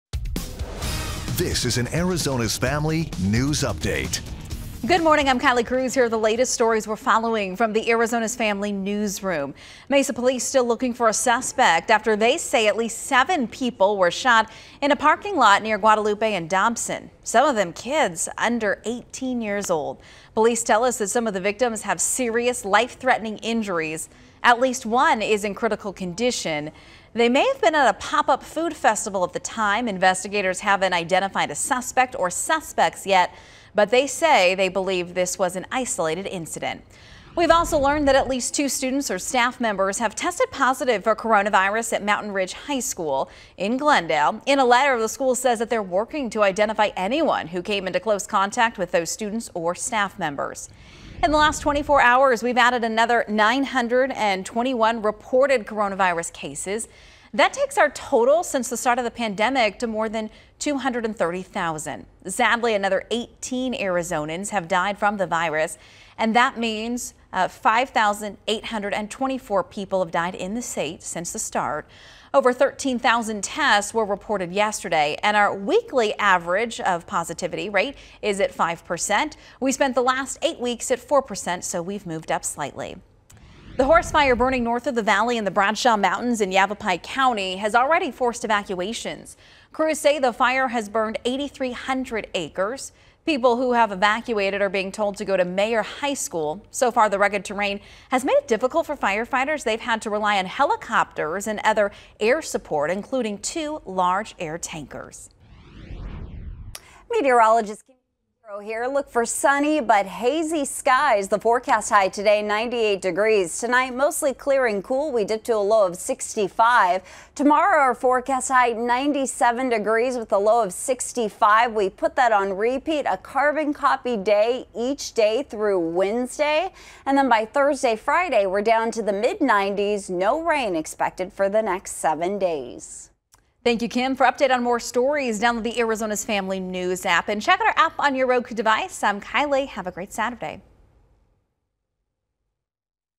News Update: Children among 7 wounded in Mesa shooting, police say; Another Maricopa County school
Seven people, some of them children, were shot in a Mesa parking lot Friday night, police said Saturday morning. At least two students or staff members at Mountain Ridge High School in Glendale have tested positive for COVID-19. Maricopa County says there now 26 active coronavirus outbreaks in schools.